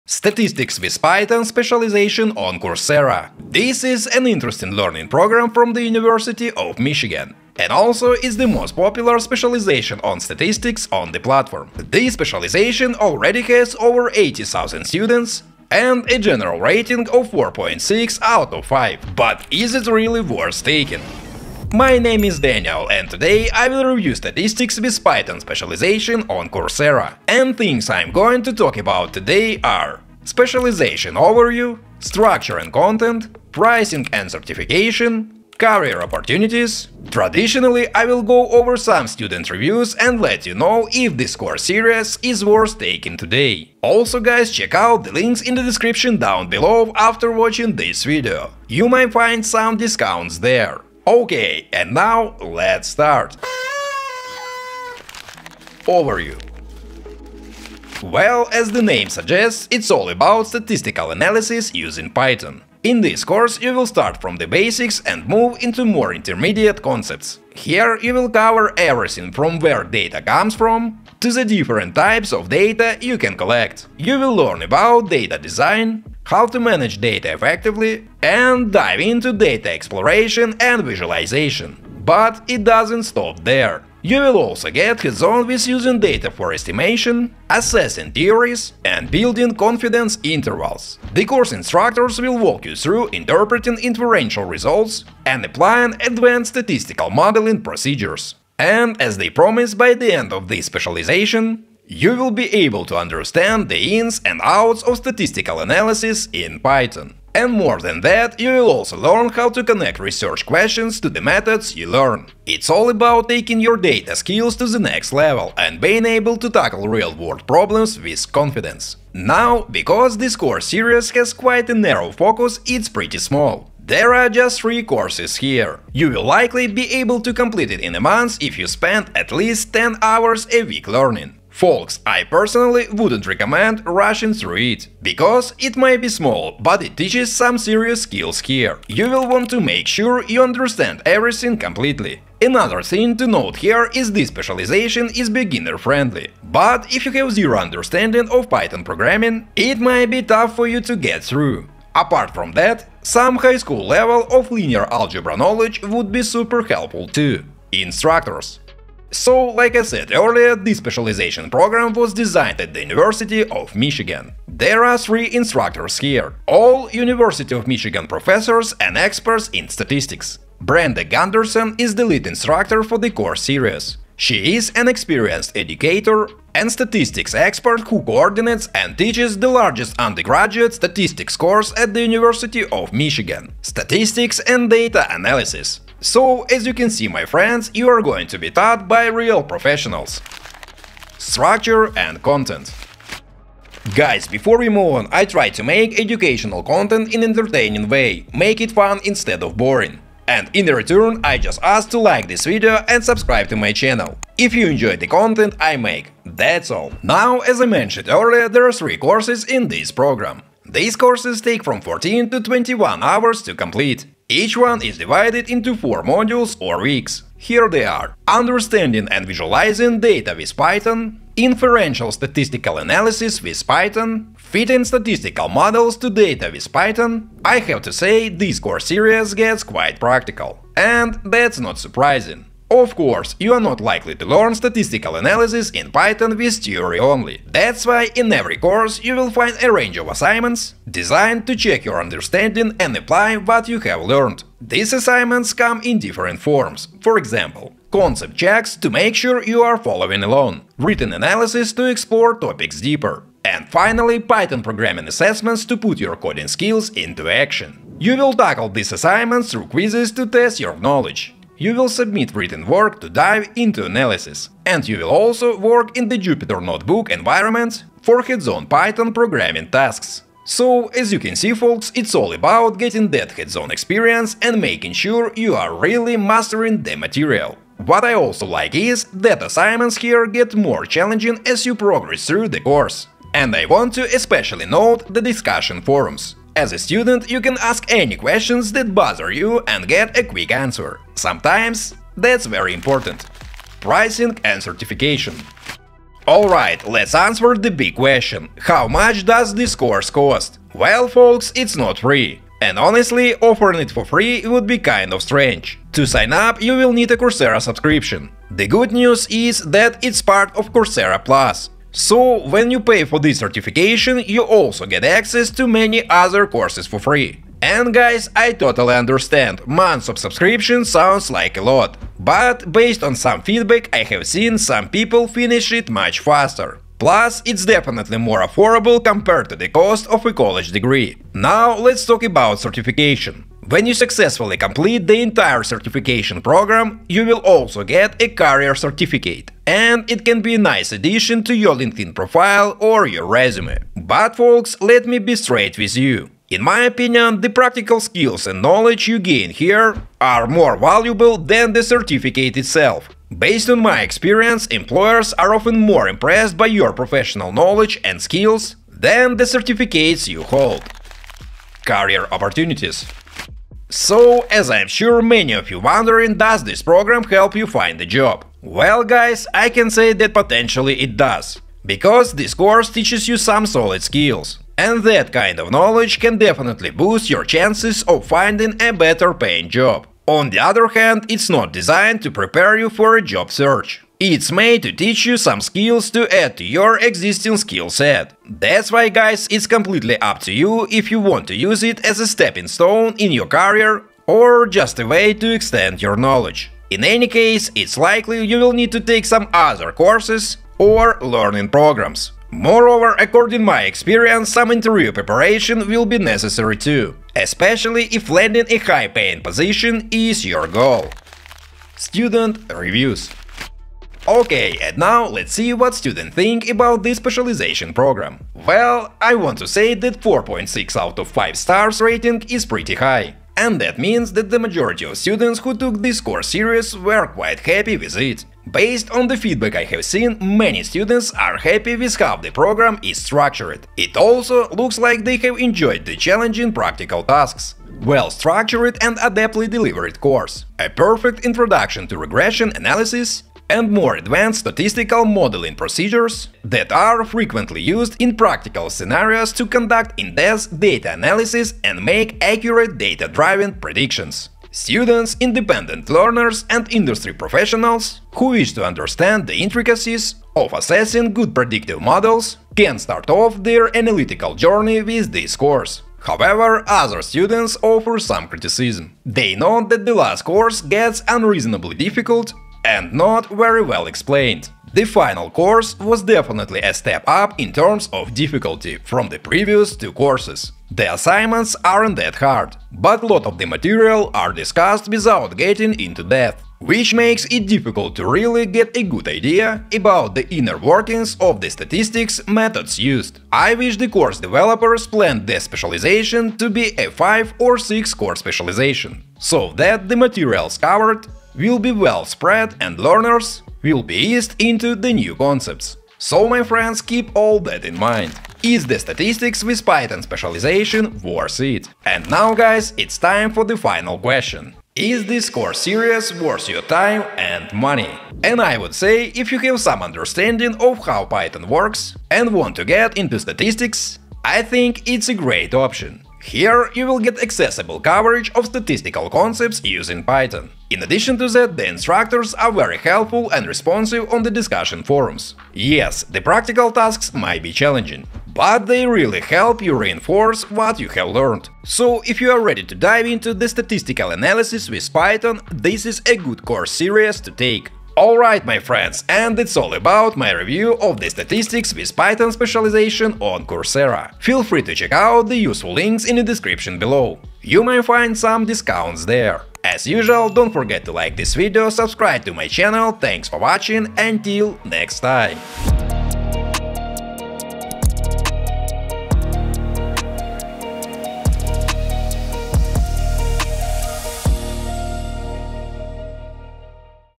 Statistics With Python Specialization Review - 2025 (University of Michigan)
Today, I'll be discussing the Statistics With Python Specialization by the University of Michigan. This course series is based on Coursera. I'll cover the overview of the specialization, its structure and content, pricing and certification, career prospects, and finally student feedback. Plus, I'll share my own insights with you.

⏱️CHAPTERS:
0:00 Statistics With Python Specialization Review
0:57 Overview
2:38 Instructors
3:12 Structure and Content
4:55 Pricing and Certification
6:03 Career Opportunities
6:52 Student Reviews
8:30 Is Statistics With Python Specialization Worth it?